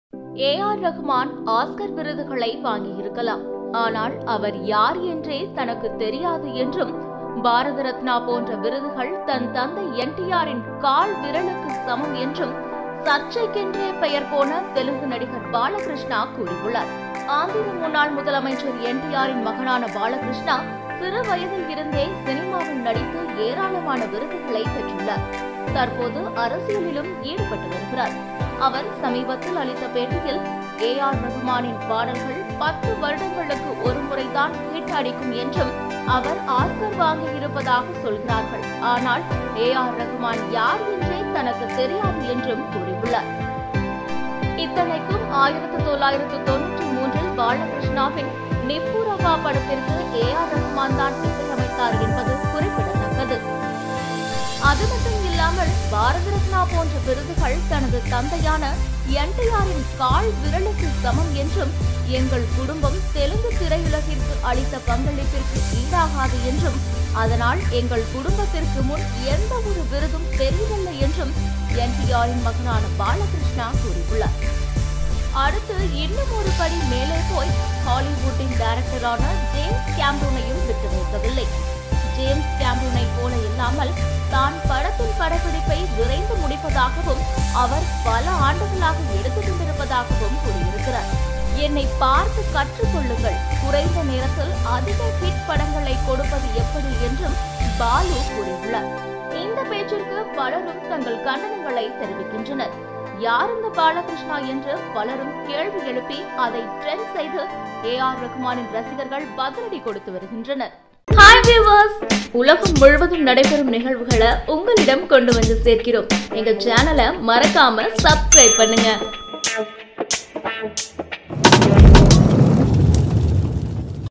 ஏன் ரஹ்மான் தனக்கு யாரென்றே தெரியாது என்று கூறுகிறார்பாலகிருஷ்ணா?
A.R.ரஹ்மானை தெரியாது
திமிரா பேசும் தெலுங்கு நடிகர்